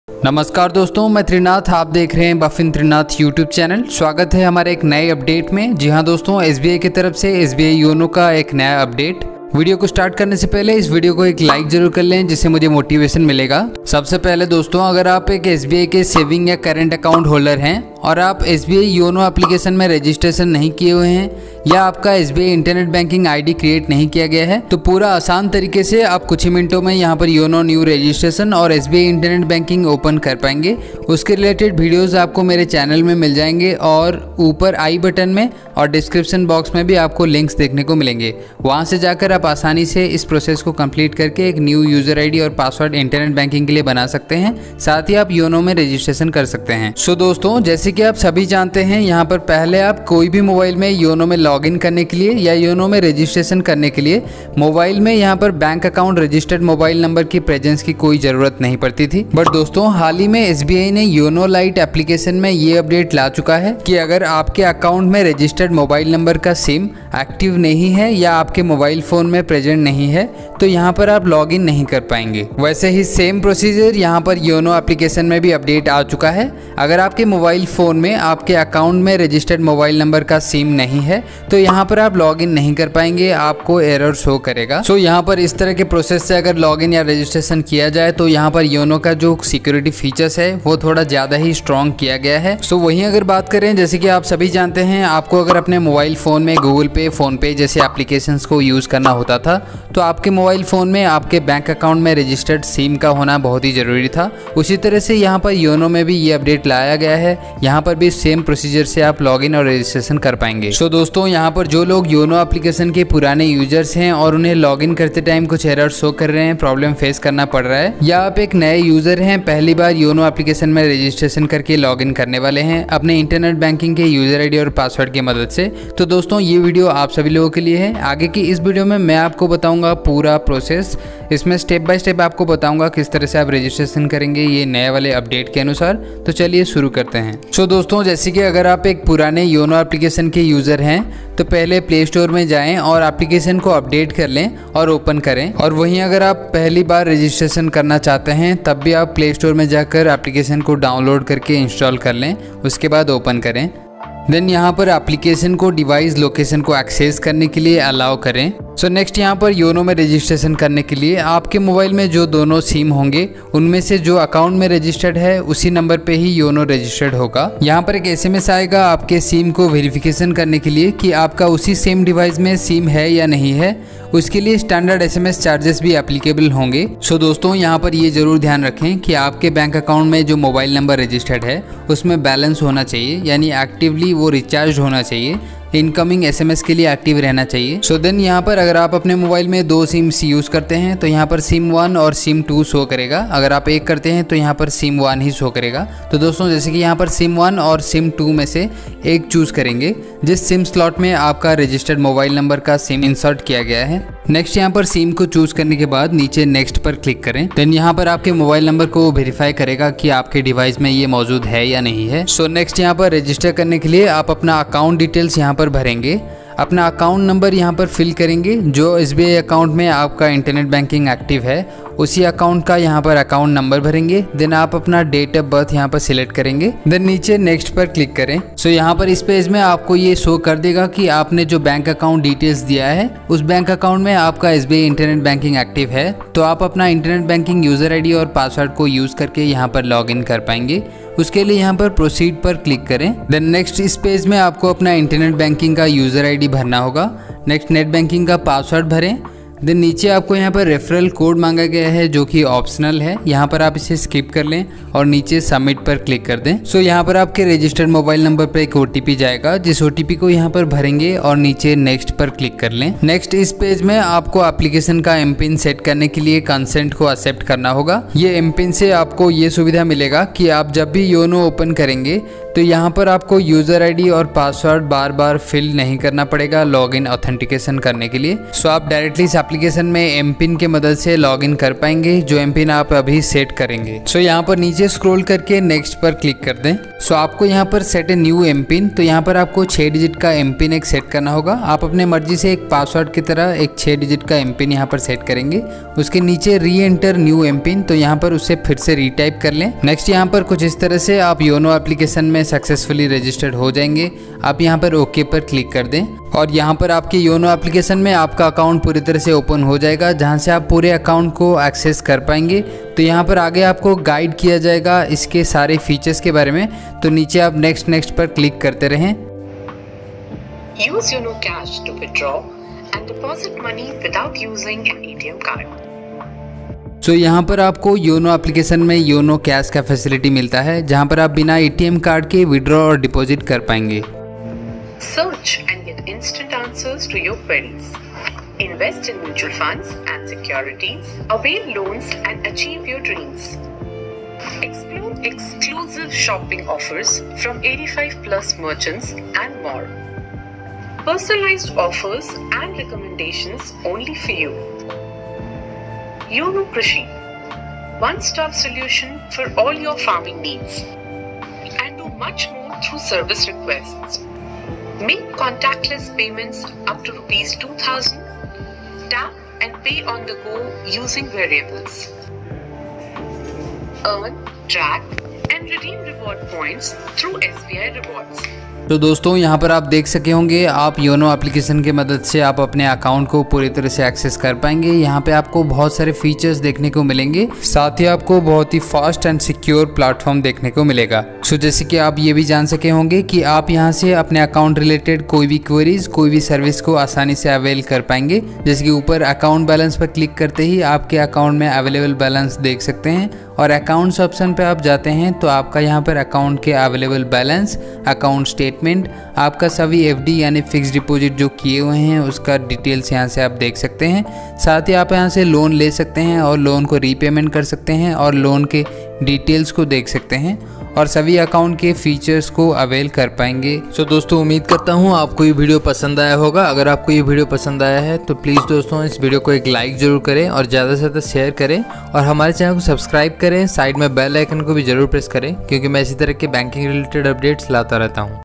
Yono SBI Registration New Process 2021 | SBI Yono Login Update | New Update Sim Login
Hello Friends,
Welcome to Boffin Trinath
In This Video:
You will get know solution of every error during yono login | New User Yono Registraion | New Updated sim login  Process

Yono SBI is leveling up its security features. The new upgrade will allow access to YONO SBI only from the phone which has the mobile number registered with the bank.